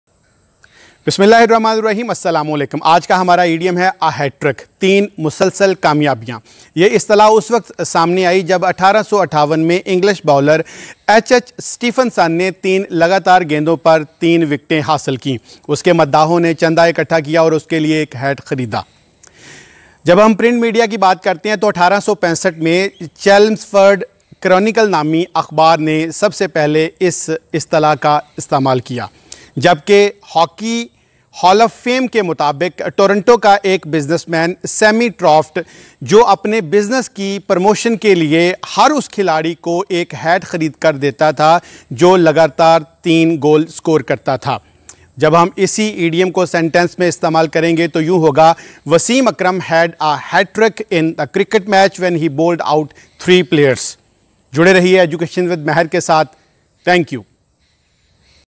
A hat-trick|idioms|Ep#15|Javaid Mehar
History and explanation of an idiom "A hat trick".